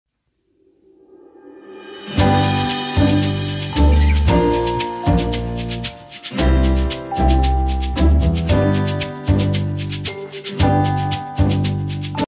Origami Nazgul (Jason Ku) Reupload
Folded from 60x60 cm Kraft
Re-uploaded as I have now added wire.